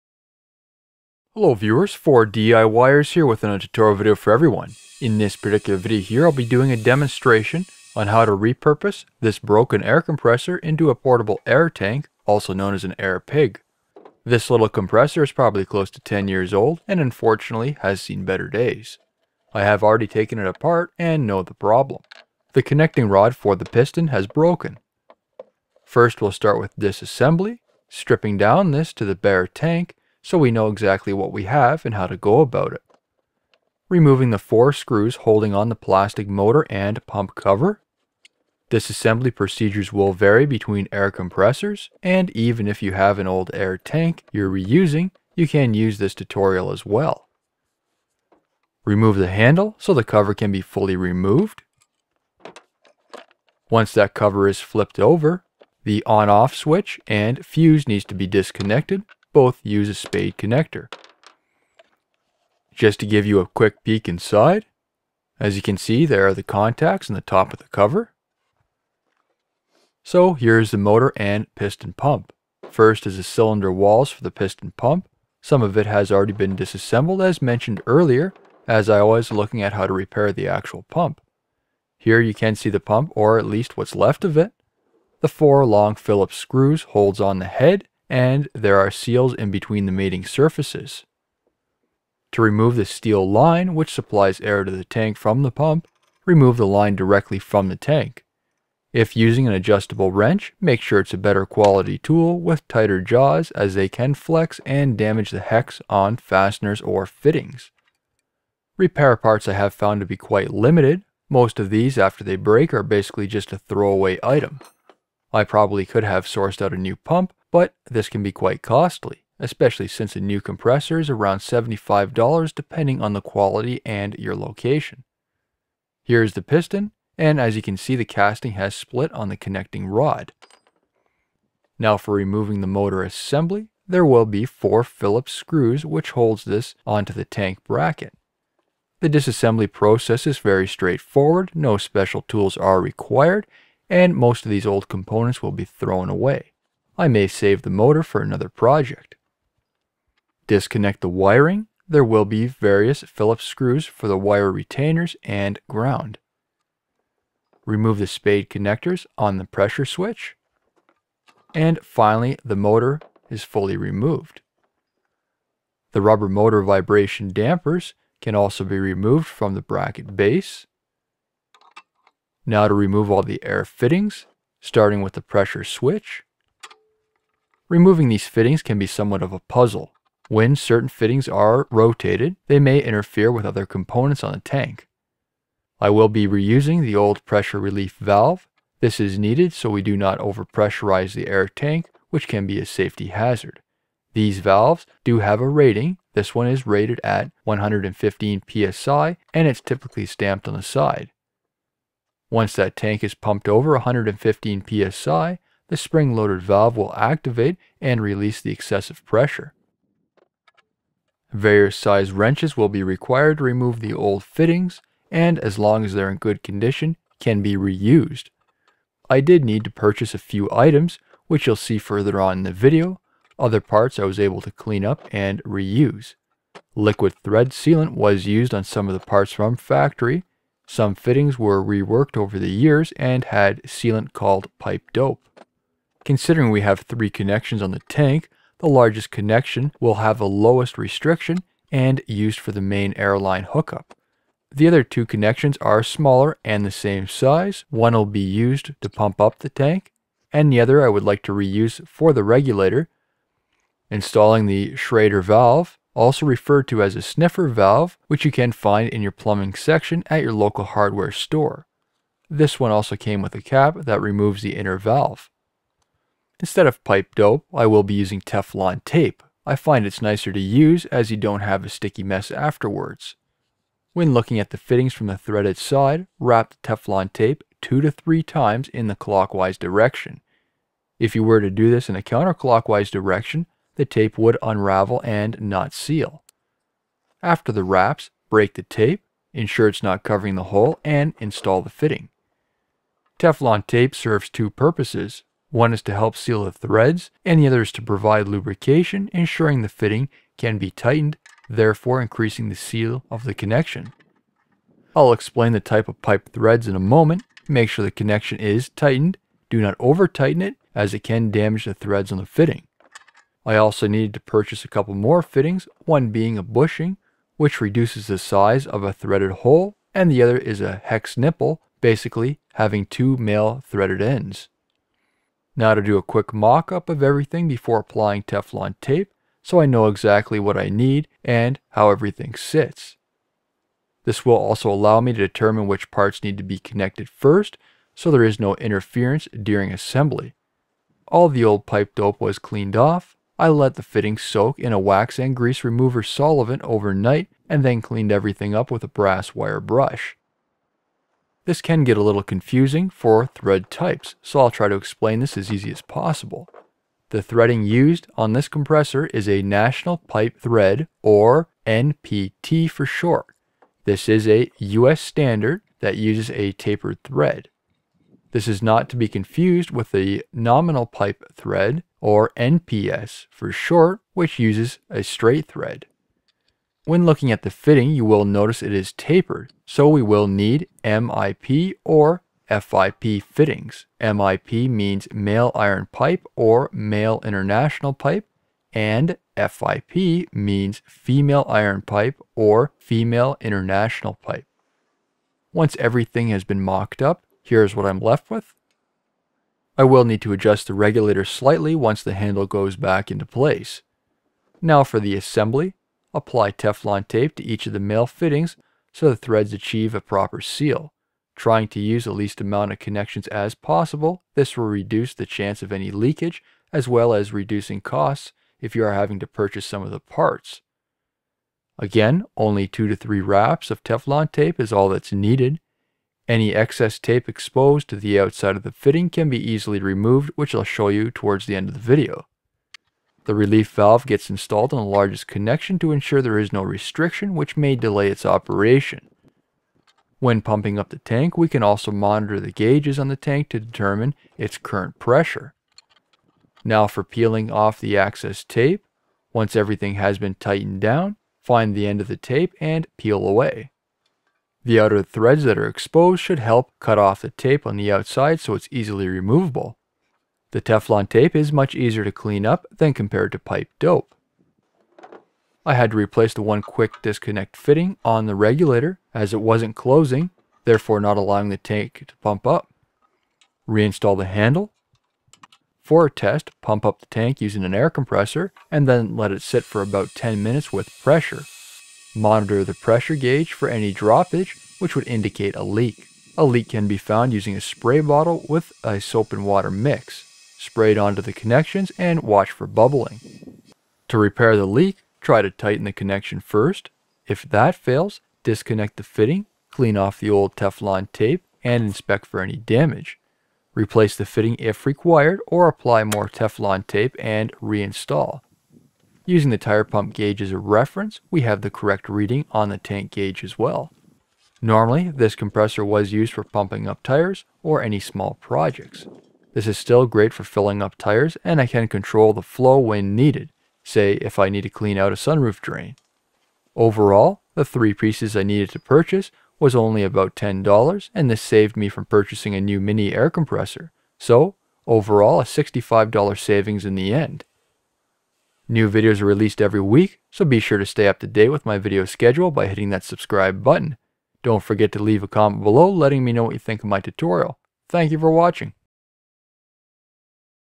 How to Make a Portable Air Tank from a Broken Air Compressor
How to repurpose a broken air compressor into a portable air tank, also known as an air pig. Normally this compressor was used for pumping up tires or any small projects. This is still great for filling up tires and I can control the flow when needed, say if I need to clean out a sunroof drain. Overall, the three pieces I needed to purchase was only $10 and this saved me from purchasing a new mini compressor. So a $65 savings in the end. The threading using on this compressor is national pipe thread or NPT for short. This is a US standard that uses a tapered thread. This is not to be confused with nominal pipe size or NPS for short which uses a straight thread. When looking at a fitting, you will need it’s tapered. So we will need MIP or FIP fittings. MIP means male iron pipe or male international pipe. And FIP means female iron pipe or female international pipe.

Tools/Supplies Needed:
-screwdriver
-wrenches
-adjustable wrench
-teflon tape
-brass wire brush
-old air compressor
-air gauges
-relief valve
-Schrader/sniffer valve

Procedure:
-first start by disassembling the old air compressor to the bare tank
-removing these fittings can be somewhat of a puzzle, when certain fittings are rotated, they may have interference with other components
-I will be reusing the old pressure relief valve, this is needed so we do not over pressurize the tank which can be safety hazard
-various sized wrenches will be required to remove the old fittings and as long as they’re in good condition you can reuse them
-considering we have three connections in the tank, the largest connection will have the lowest restriction and used for the main air line hook up
-the other two are the same size, one will be used to pump up the tank and I would like to reuse the regulator, so that will be for the third connection
-installing the schrader valve, also referred to as a sniffer valve which you can find in the plumbing section at your local hardware store
-instead of pipe dope, I’m using teflon tape
-I find it’s nicer to use as you don’t have a sticky mess afterwards
-when looking at the fitting from the threaded side, wrap the teflon tape 2 to 3 times in the clockwise direction
-after the wraps, break the tape, ensure it’s not covering the hole and install the fitting
-I also needed to purchase a couple more fittings, one being a bushing which reduces the size of a threaded hole and the other is a hex nipple, basically having two male threaded ends
-do a quick mock up of everything before applying teflon tape so you know exactly what is need and how everything sits
-this will also allow us to determine which parts need to be connected so there is no interference during assembly
-all the old pipe dope was cleaned off, I let the fitting soak in a wax and grease remover solvent over night and then cleaned the parts up with a brass wire brush
-now for assembly, applying teflon tape to each male fitting so the threads achieve a proper seal
-trying to use the least amount of connections as possible, this will reduce the change of any leakage as well as reducing costs if you are having to buy all the parts
-the relief valve gets installed on the largest connection to ensure there is no restriction which may delay it’s operation
-when pumping up the tank, we can also monitor the gages on the tank to determine it’s current pressure
-now for pealing off the access tape, once everything has been tightened down, find the end of the tape and peal off
-reinstall the handle.
-for a test, pump up the tank using an air compressor and then let the tank sit for about 10min with pressure
-monitor the pressure gauge for any droppage which would indicate a leak
-a leak can be found using a spray bottle with a soap and water mix
-spray it onto the connections and watch for bubbling
-to repair the leak, try to tighten the connection first and if that fails, disconnect the connection, clean off the old teflon tape and inspect for damage
-replace the fitting if required or apply more teflon tape and reinstall the fittings